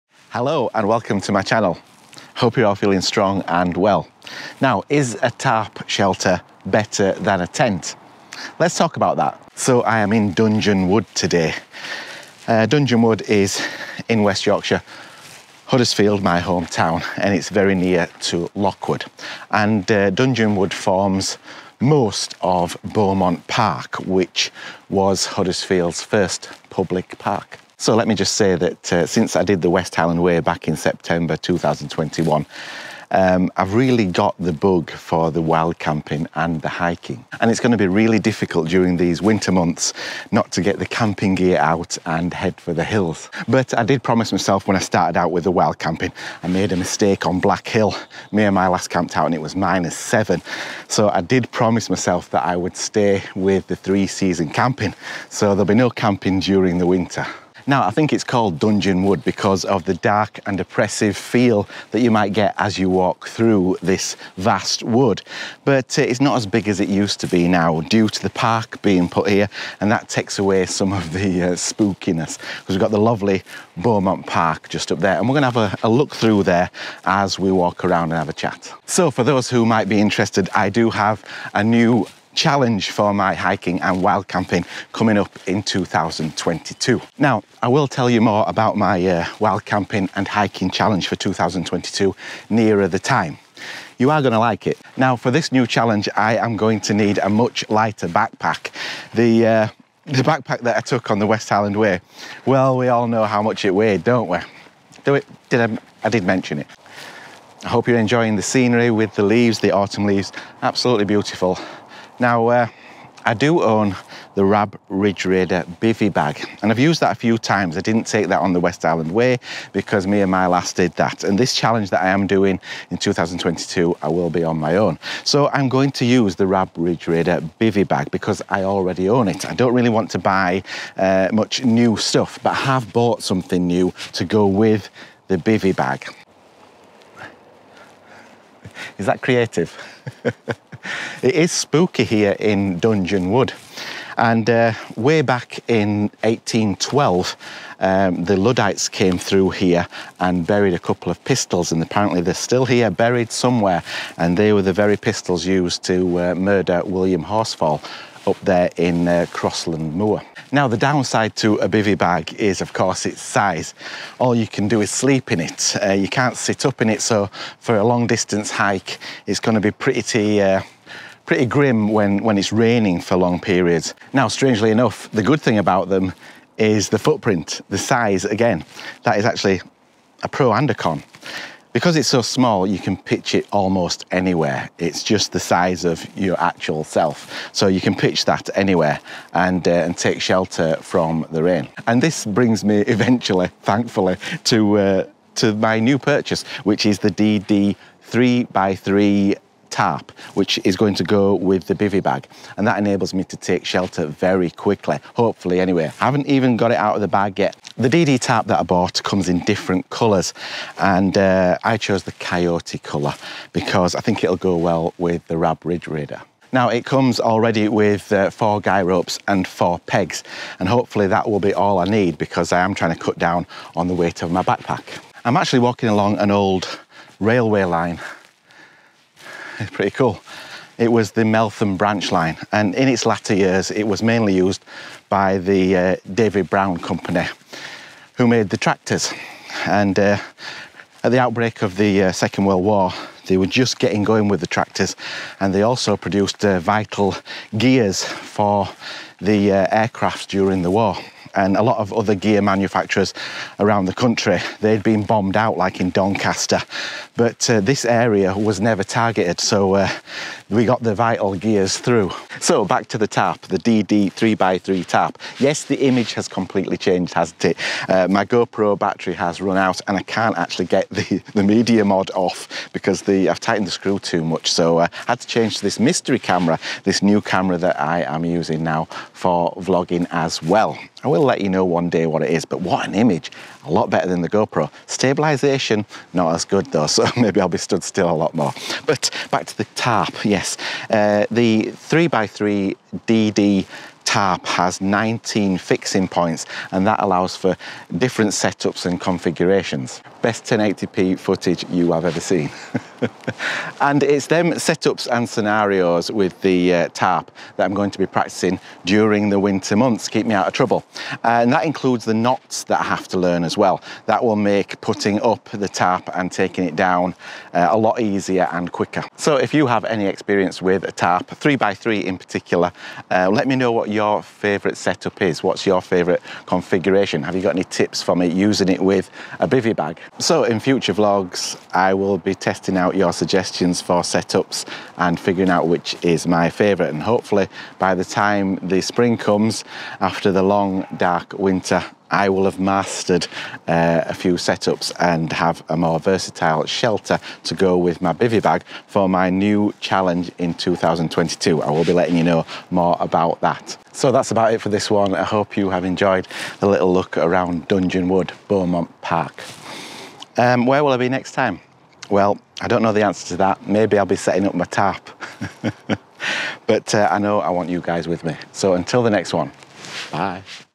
Tarp & Bivvy - For wild camping?
I am here in Dungeon Wood situated to the south-west of Lockwood in Huddersfield West Yorkshire and forms a major part of Huddersfield's first public Park Beaumont Park.
So I just want to let you know that after doing the West Highland Way Back in September 2021 I have really got the bug for wild camping and hiking.
It's very difficult during these winter months not to get the camping gear out and head for the hills
But I promised myself After enduring a freezing night - 7 degrees On Black Hill that I would stick to being a three season camper.
its called dudgeon wood due to the dark oppressive feel you might get as you walk through
that was of course way before this beautiful Victorian park was built.
So for those who might be interested I do have a new new  hiking and camping challenge for 2022
 I will tell you more about that nearer the time haha keeping you in suspenders
For this new challenge I am going to need a much lighter backpack
I already own a Rab Ridge Raider bivvy bag
 so I am going to make use of that
In 1812, the Luddites who shot William Horsfall fled through the wood from Crosland Moor, and according to some reports, two of their pistols were buried there
Shortly before the outbreak of the Second World War, the firm of David Brown had begun production of tractors at Meltham Mills. During the war, the company also manufactured vital aircraft gears. Whilst other gear factories at Coventry and Derby were repeatedly bombed by the Luftwaffe, the David Brown works at Lockwood and Meltham Mills were never targeted.
Following the end of the war, David Brown's soon became the primary user of the branch line.
The downside to a  bivvy bag Is of course it's  size
 you can't sit up in it
 all you can do is sleep in it
The good thing about them is the small footprint
 this will enable me to pitch up almost anywhere
To quickly Take Shelter from the rain
 I am going to use this
  the DD 3 x 3 tarp
I chose the coyote colour maybe to go with my Rab Ridge Raider
It comes with 4 guy ropes and 4 pegs
 it has 19 attachment points this allows for multiple setups and configurations
 and it's those configurations and setups that I'm going to practice during the winter months
 this includes the  knots that I will need to learn turn to make setting up and taking down fast and easy
So if you have any experience using a tarp  let me know what your favourite configuration is
Have you used a tarp setup with a bivvy bag?
Outro
So that's it for this one Hope you can help me out As you have always done before
Where will I be next time?
in future blogs I will be trying out some of your suggestions
And if all goes well I will bring the weight of my backpack down
And hopefully have a more versatile shelter For my next challenge in 2022
I hope you're going to join me again in my next video
 or of course I would be all by myself
 so until the next one bye